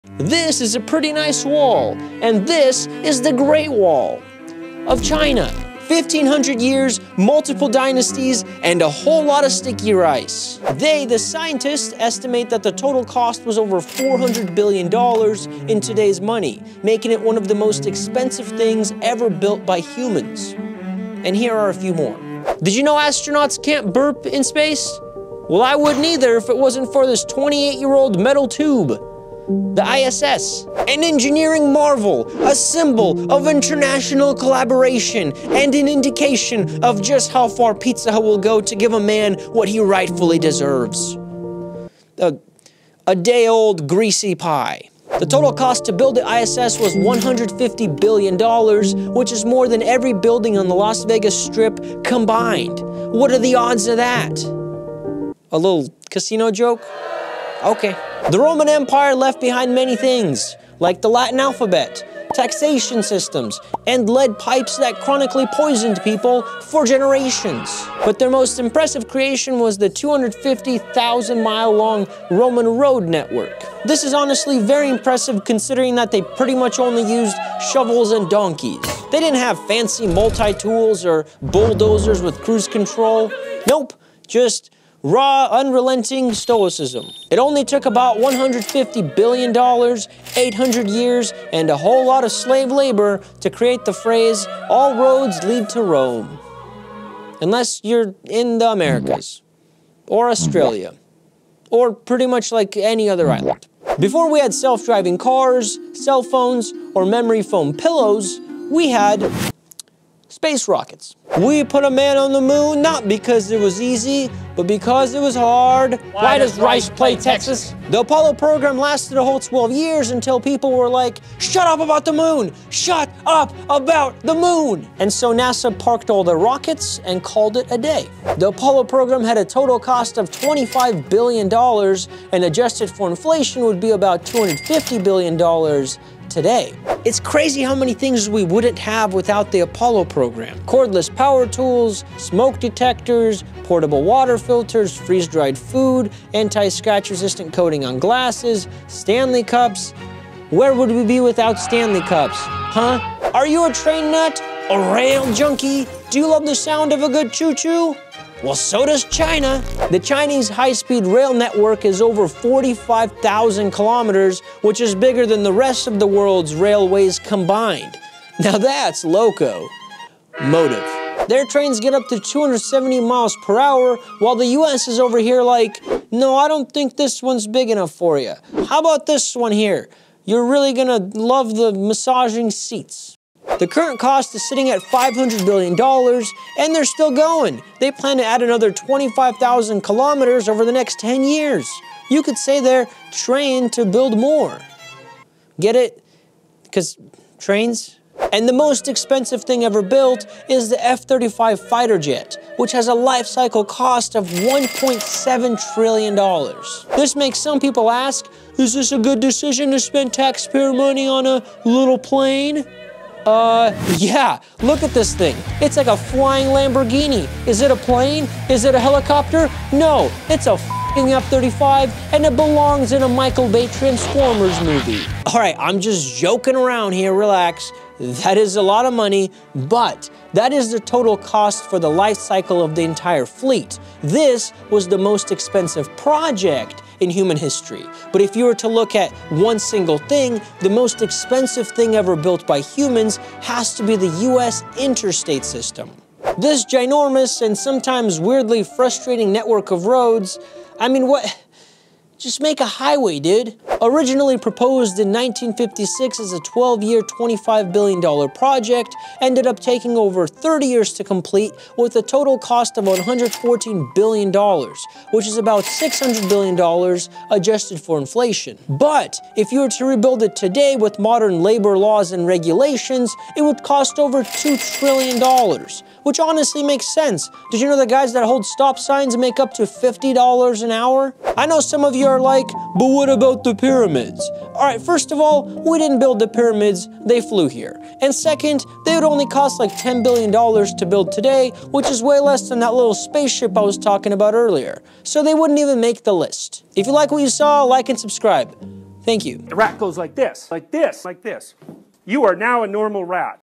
The Most Expensive Things Ever Built By Humans
From ancient wonders to modern megaprojects, this video dives into the most expensive things ever built in human history… massive undertakings that cost more than some countries’ entire economies. We’ll explore how they were made, who paid for them, and what drove humanity to pour unimaginable resources into these colossal creations. From military projects to space exploration and monumental architecture, these builds reveal what happens when ambition, power, and technology collide on a global scale.

Chapters
0:00 Intro
0:22 ISS
0:58 Roman Empire
1:44 Apollo Program
2:32 China Rails
3:17 F-35
4:05 Interstate System
4:41 Triangles